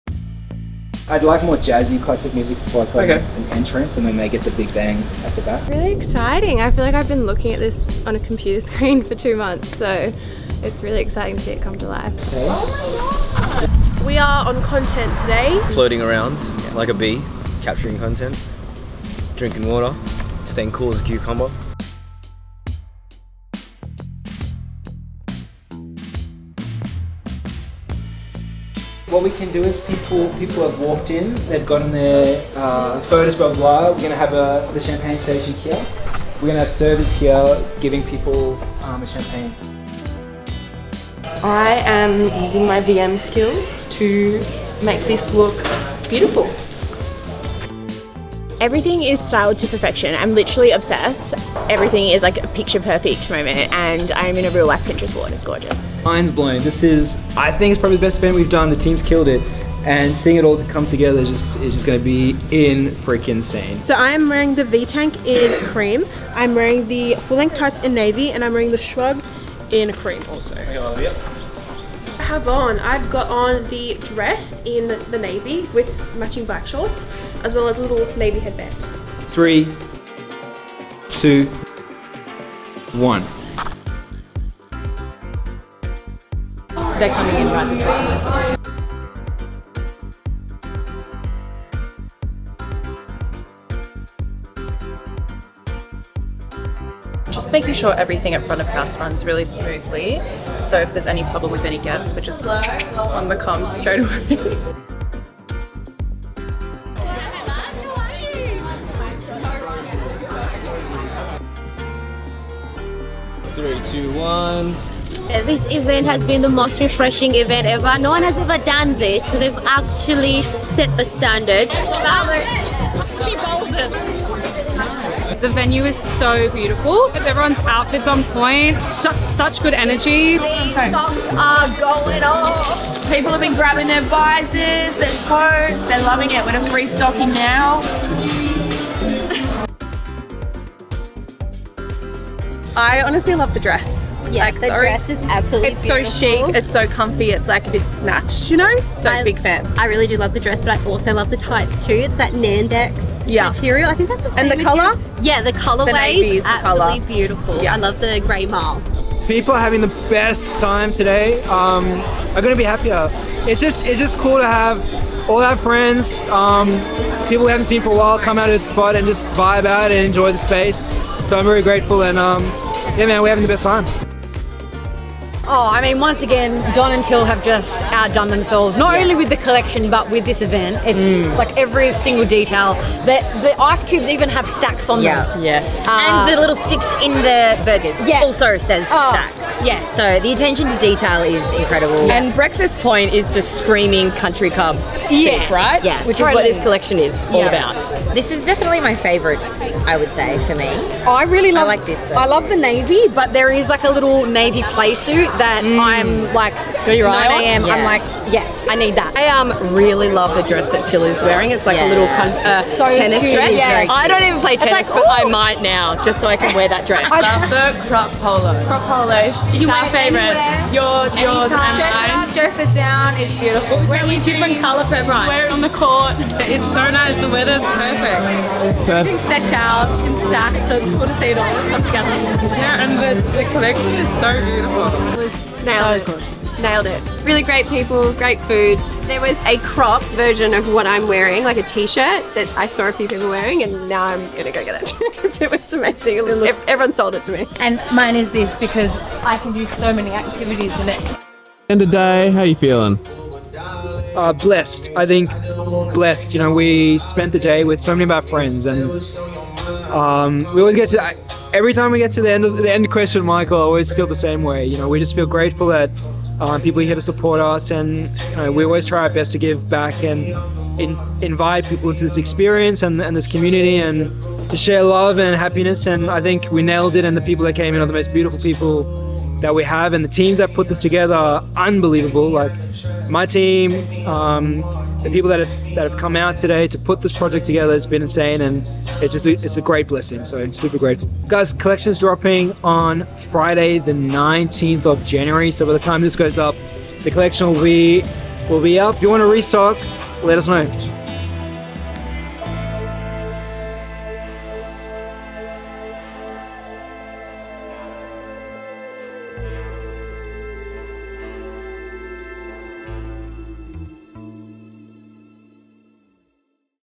The Country Club Event of the Season | STAX. BTS
Join the STAX. team and the closest friends of the brand at the Country Club event of the season! Make sure you start your year off right wearing the Racquet Club collection available online and in store now!